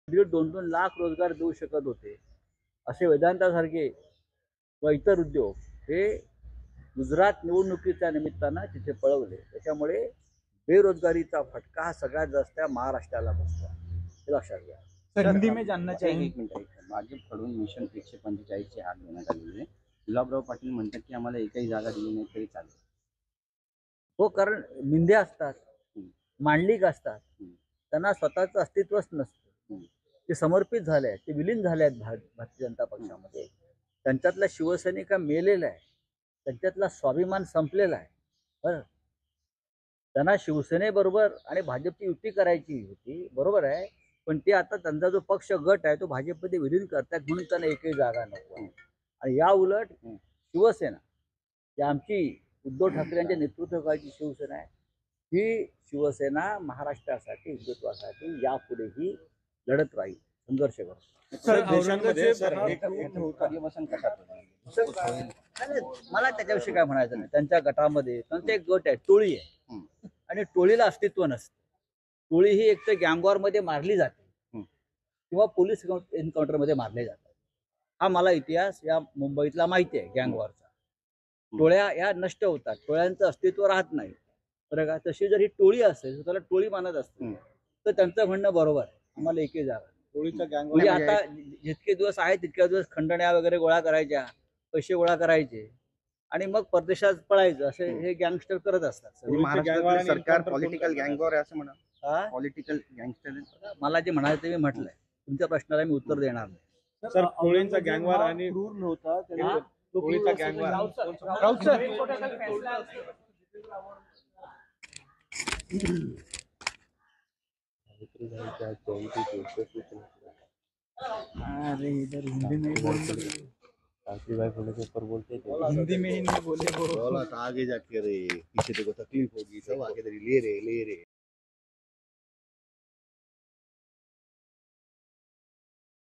MP Sanjay Raut on Shinde : इथले प्रकल्प गुजरातला नेले त्यामुळे सर्वात जास्त बेरोजगारी महाराष्ट्रात
बेरोजगारीच्या दरात वाढ हे प्रामुख्याने शहरी भागातील बेरोजगारी वाढल्यामुळे आहे. (sanjay raut criticized on shinde fadanvis government ) यावर आता खासदार संजय राऊत यांनी भाष्य केले असून शिंदे फडणवीस सरकारवर टीका केली आहे. ( employment generating projects were taken to Gujarat ) वेदांता सारखे व इतर उद्योग हे गुजरात निवडणुकीच्या निम्मितान तिथे पळवले. त्यामुळे बेरोजगारीचा फटका सर्वात जास्त महाराष्ट्राला बसला आहे. (outgoing projects from Maharashtra to Gujarat)